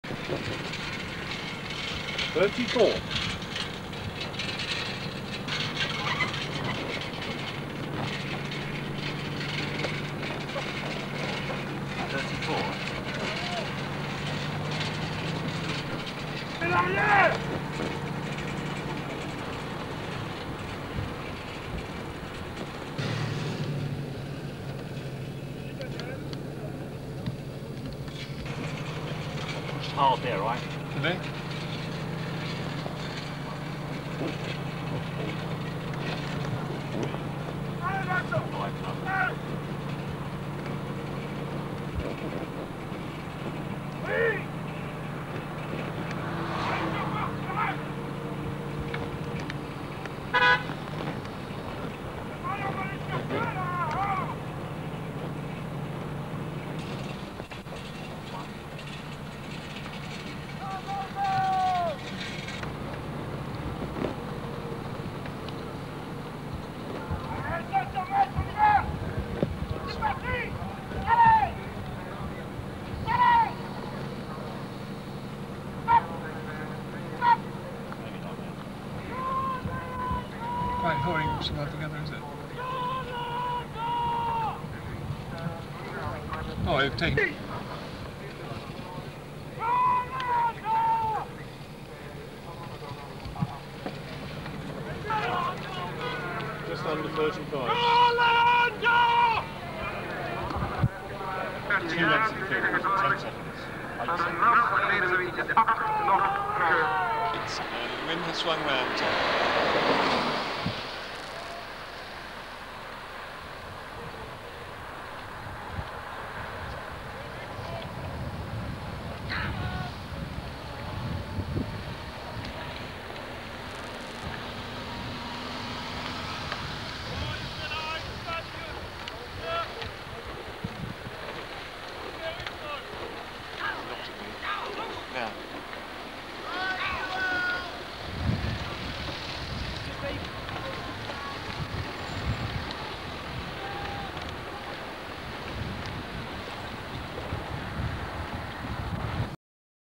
1985 Ghent mens 4- final
Saturday Tyrian (GB) 6:33.2 beating Leander 6:41.3, Molesey 6:44.0, French U23 crew 6:45.8 and Imperial College 7:19.4
Sunday losing to 1983 World Champions from West Germany (Hansa Dortmund/Witten).  Final result Hansa Dortmund/Witten 6:30.6, Tyrian 6:32.9, Molesey 6:46.2, W German composite 6:47.3, French composite 7:15.6
The West German four went on to win the 1985 World championships as well
Copied from video taken by Rusty Williams